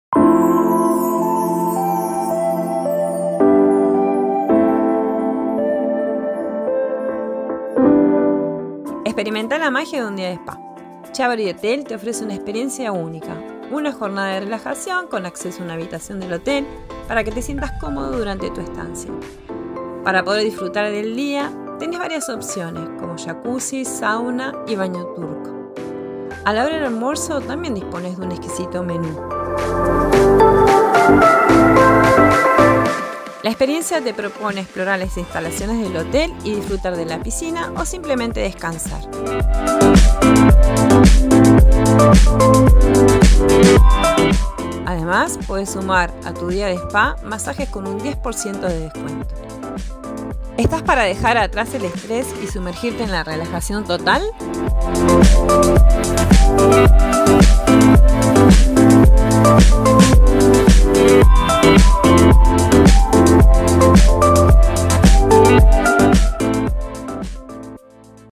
Disfrutá la magia de un día de spa
Hotel Chiavari te ofrece la posibilidad de vivir una jornada única. Jacuzzi, baño sauna, baño turco, habitación incluida y también se pueden sumar masajes.
No te quedes sin vivir la experiencia, visitalos en Chiozza N° 2260, San Bernardo.